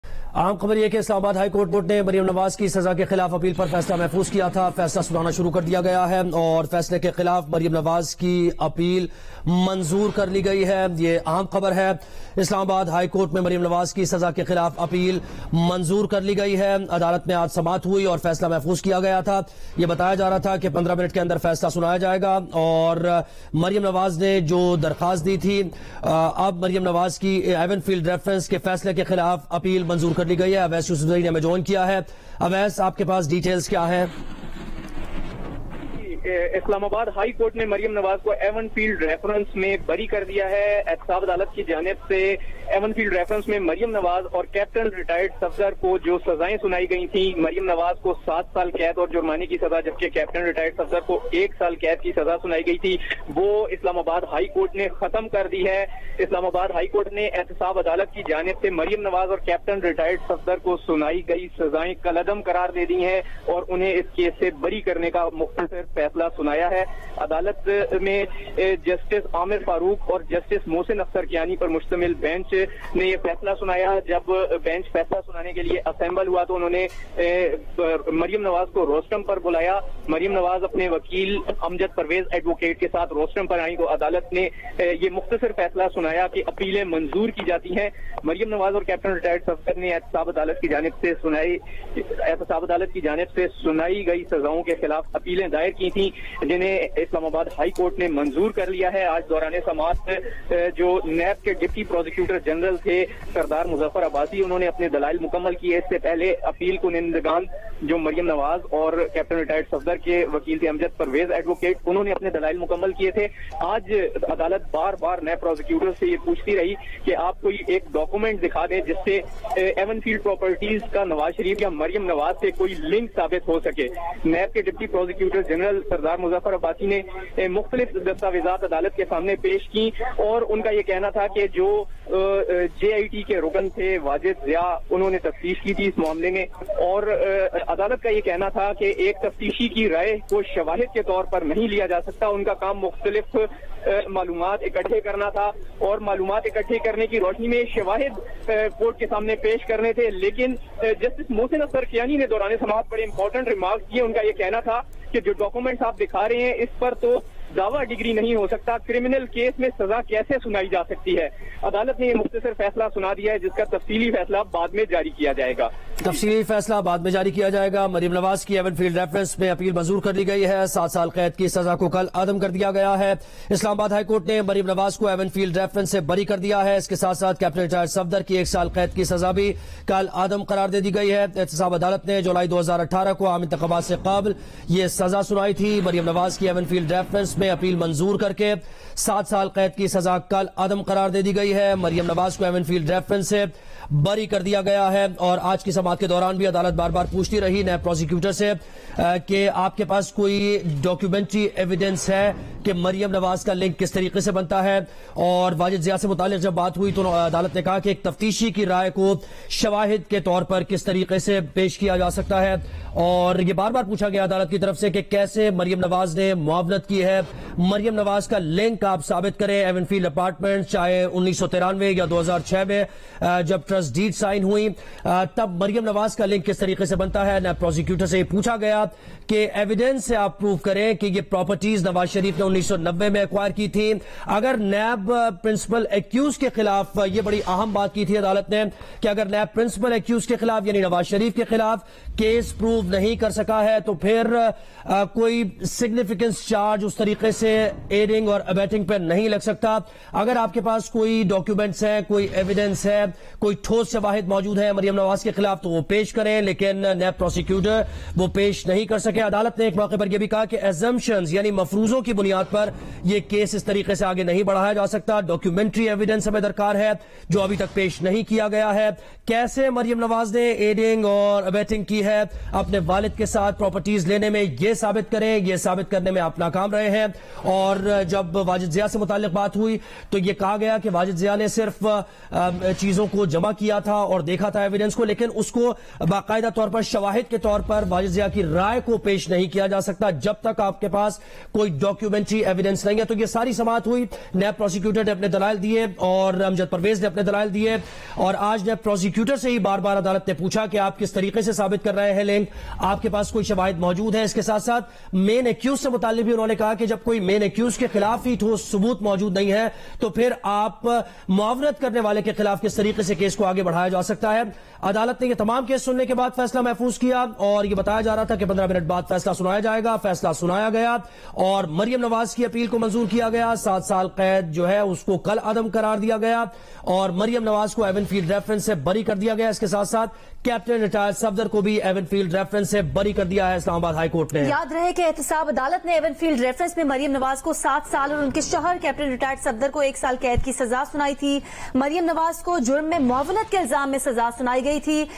IHC acquits Maryam Nawaz, Capt Safdar in Avenfield case
ISLAMABAD: The Islamabad High Court (IHC) Thursday acquitted PML-N Vice President Maryam Nawaz and her husband Captain (retd) Safdar in the Avenfield corruption reference.

A two-member bench of the IHC comprising Justice Aamer Farooq and Justice Mohsin Akhtar Kayani heard her plea.

In July 2018, an accountability court sentenced the PML-N leader to seven years in prison and her husband to one year for their involvement in the case.

The National Accountability Bureau (NAB) had accused her of abetting his father and former prime minister Nawaz Sharif in committing the crime.